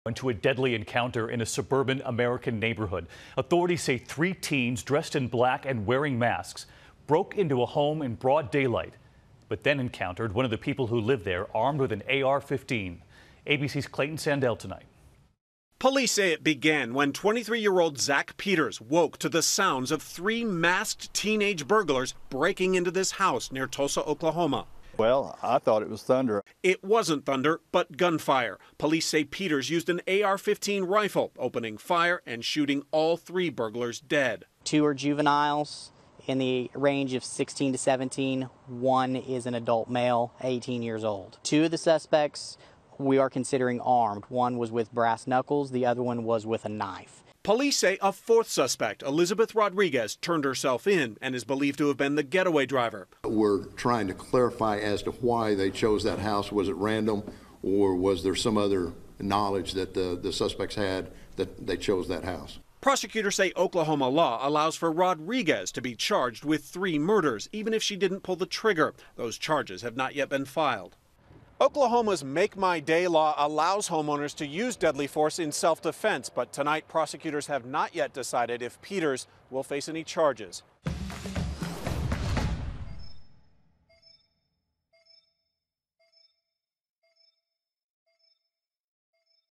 Deadly home invasion in Tulsa
Three people who allegedly broke into a home were shot and killed, and the alleged getaway driver is in custody.